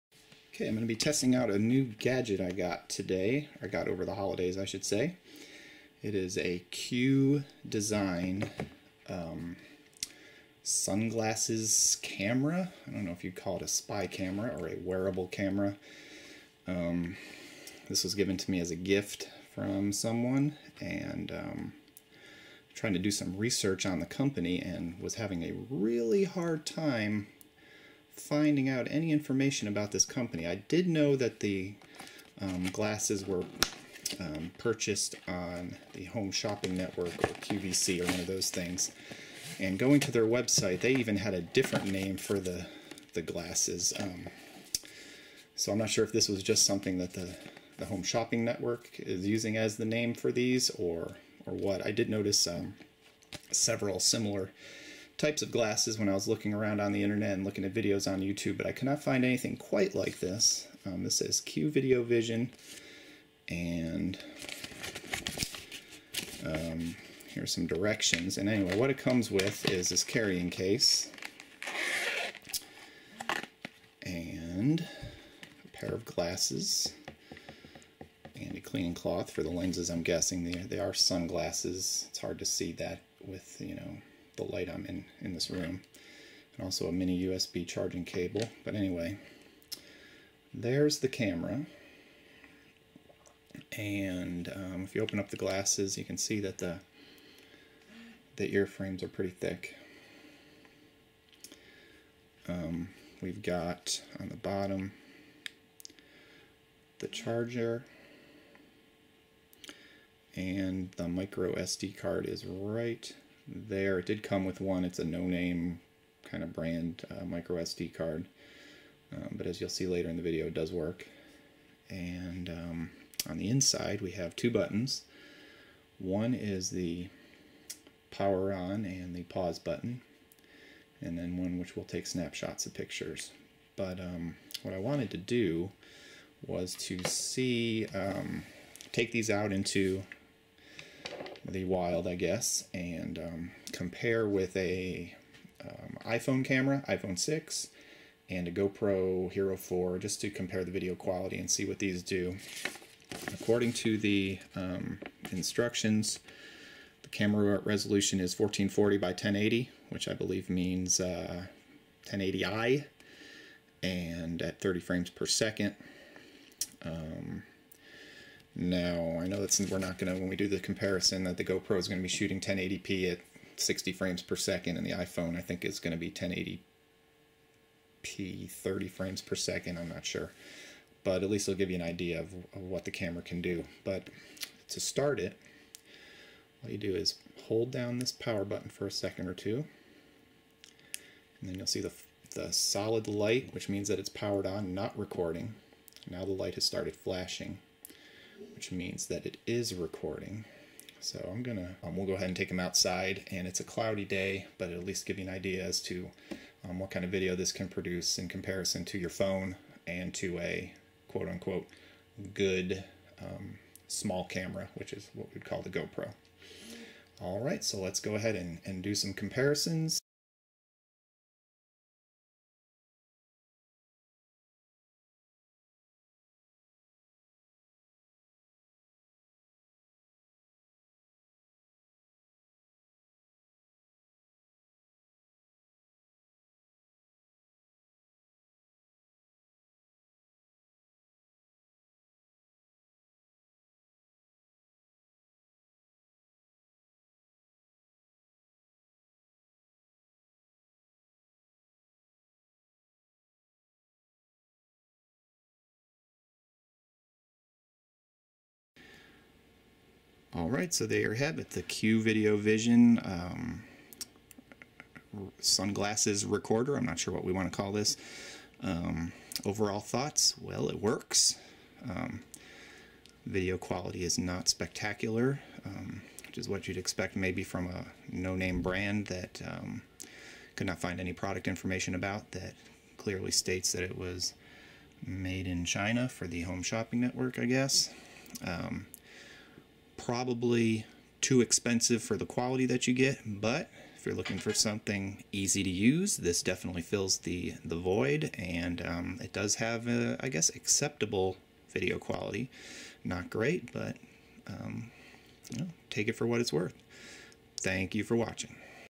GoVision HD Sunglasses Video Camera Test Comparison and Review
Sold on HSN as GoVision Polarized 1080p HD Video-Capture Sunglasses, these glasses feature 1440x1080 video and a 15 MP still camera.

In this video I will do side-by-side quality comparisons with this camera and a iphone 6 video as well as a comparison with a GoPro Hero 4 Black.